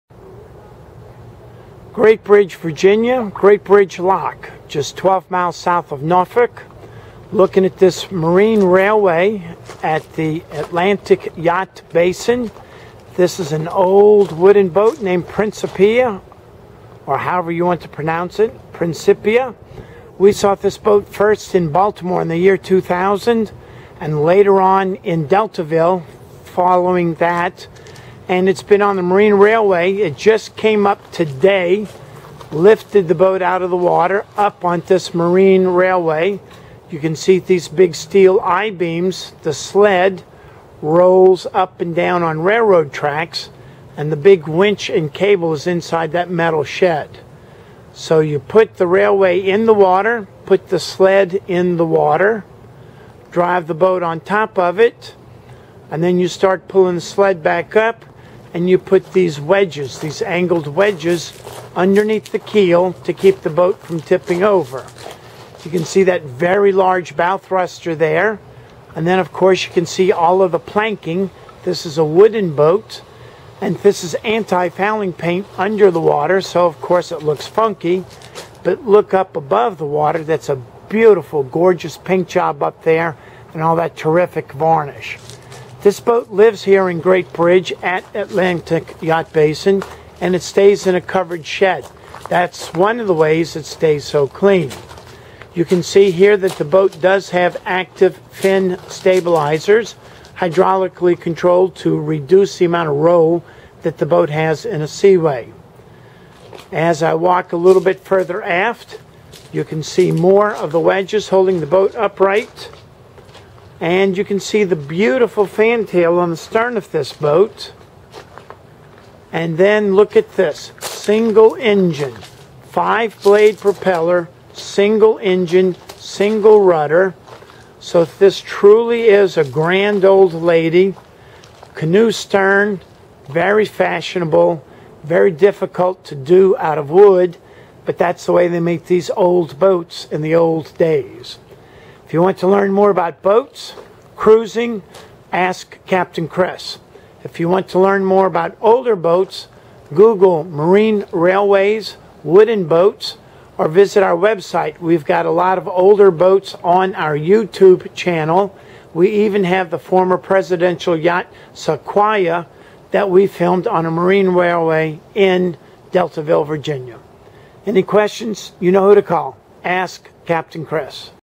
Principia on marine railway
Principia on marine railway at Atlantic Yacht Basin near Norfolk, VA. Great old woody, looks terrific.